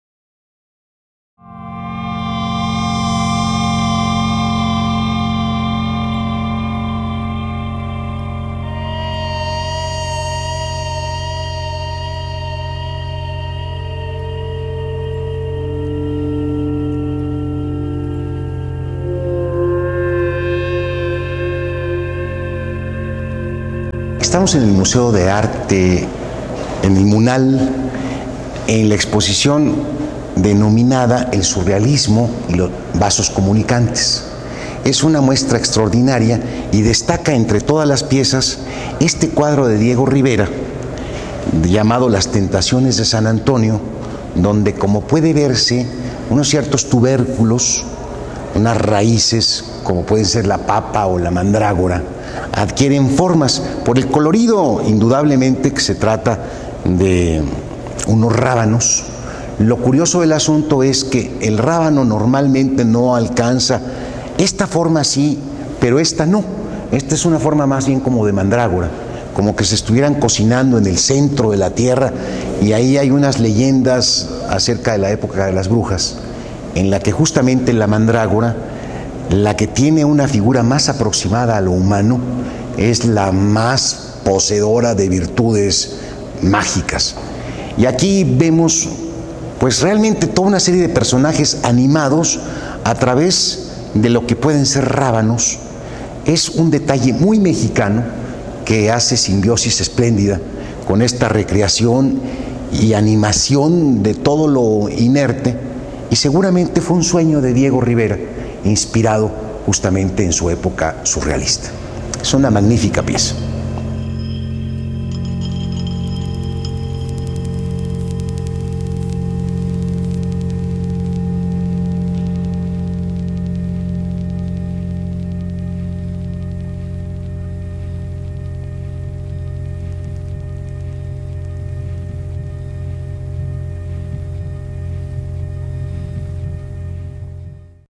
Platícame una obra. DIEGO RIVERA. Las tentaciones de san Antonio, 1947
Investigadores e historiadores del Arte nos platican una obra del acervo del Museo Nacional de Arte en México.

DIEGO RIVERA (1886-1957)
Las tentaciones de san Antonio, 1947
Óscar de la Borbolla
Filósofo